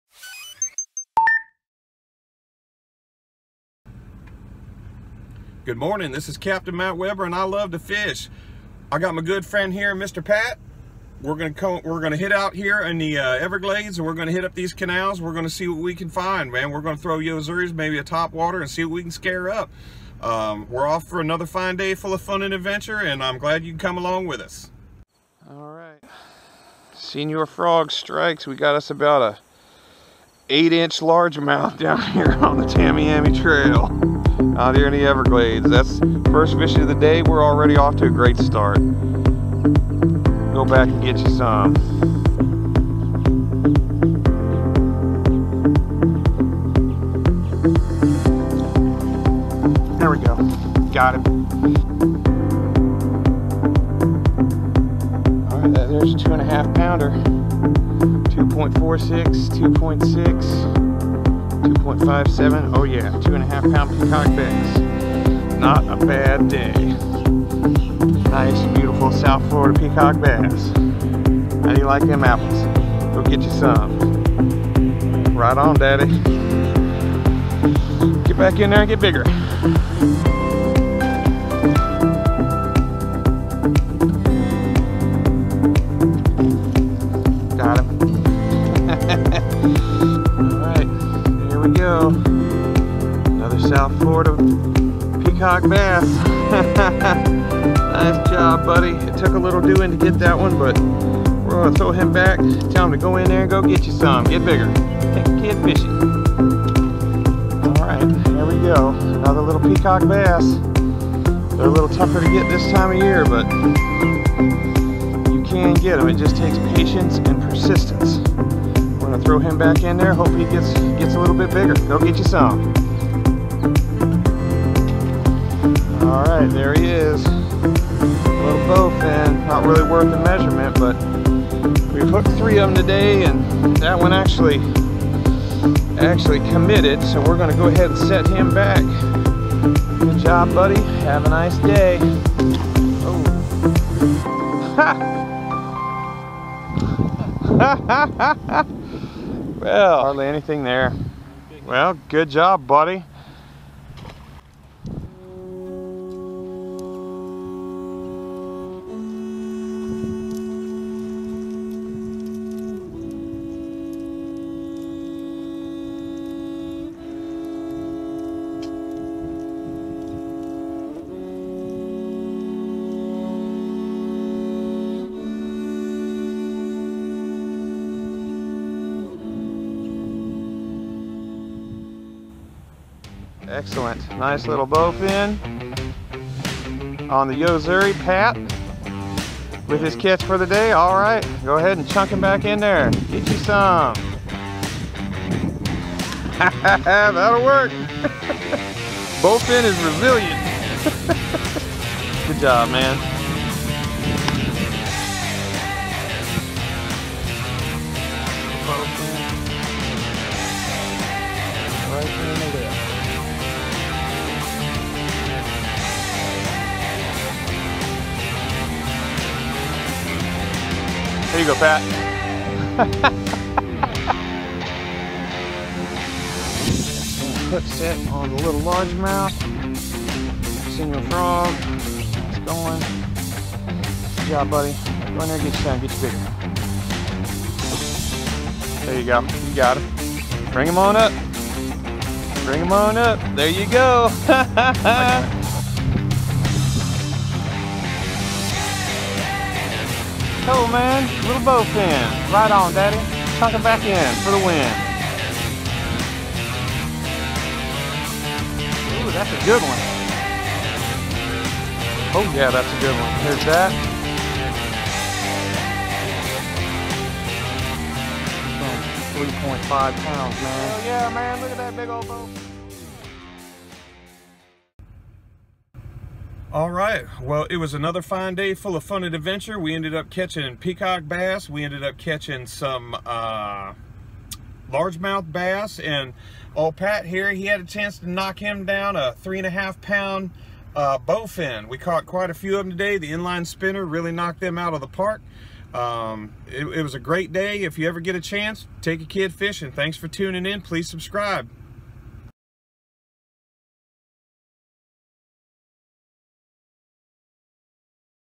Capt Matt & Pat Peacock Bass & Bowfin Tamiami Trail
My name is Captain Matt Webber and I love to fish. I took my friend Pat out the the Everglades to do a little fishing and enjoy the outdoors. We threw the Yo-Zuri, Topwater Frog and In-Line Spinner. Ended up with Pat catching several Bowfin including his Personal Best at 3.5Lbs. I managed several different types of fish including a respectable 2.5Lb Peacock Bass. It was another fine day full of fun and adventure.